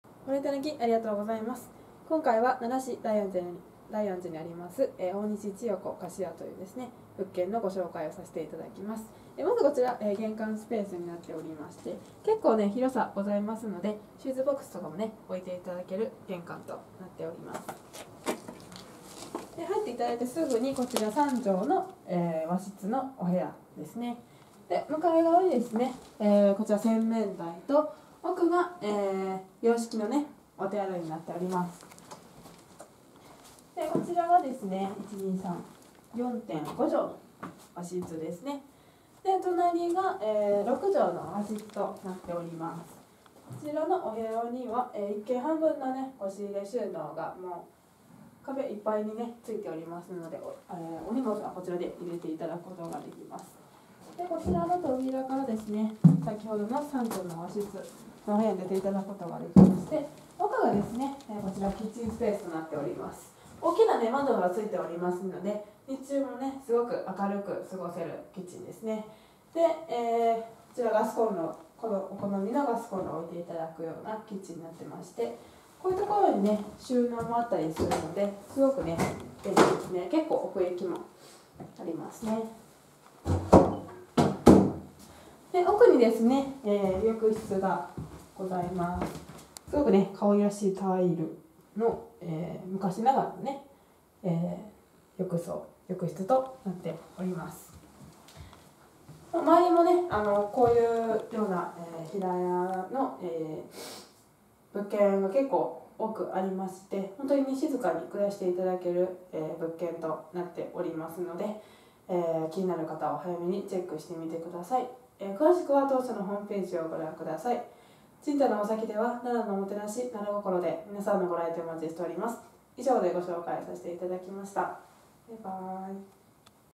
001647 大西千代子貸家♪賃貸のマサキ
奈良市大安寺５丁目にあります3Kの【大西千代子貸家】の紹介動画になります。